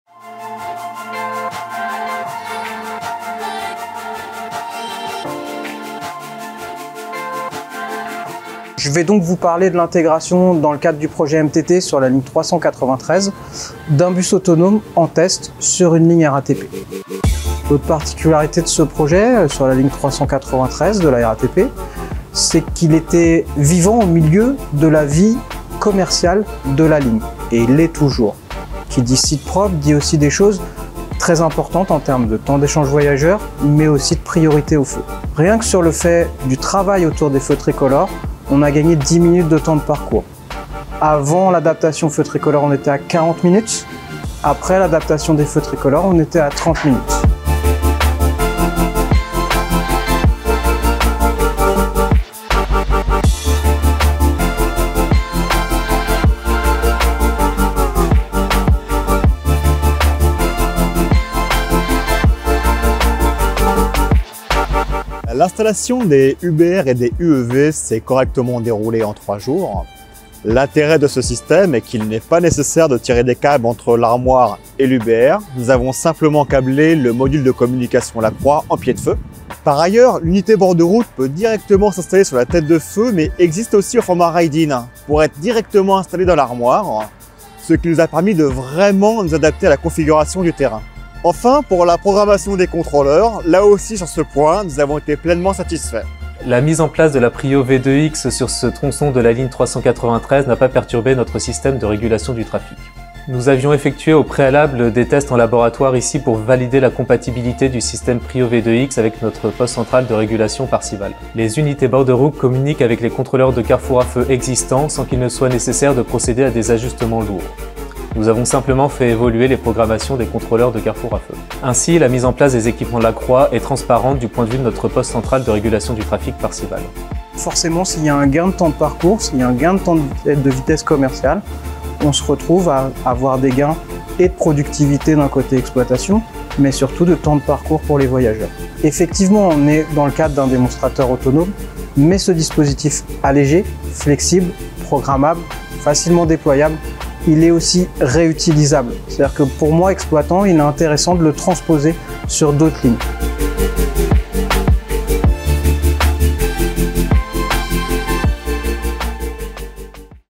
La solution Prio V2X LACROIX
La solution Prio V2X LACROIX, LE PREMIER SYSTÈME DE PRIORITÉ BUS
EVOLUTIF, INTEROPÉRABLE ET CYBERSÉCURISÉ.
Cette solution :
- Assure la régularité et optimise la vitesse commerciale des transports publics
- Sécurise la voirie partagée
- Assure la sécurité et réduit le temps de passage aux intersections pour les
véhicules d'urgence
- Accompagne le déploiement des mobilités connectées
Exemple en images avec la RATP, SERFIM T.I.C, Département du Val-de-Marne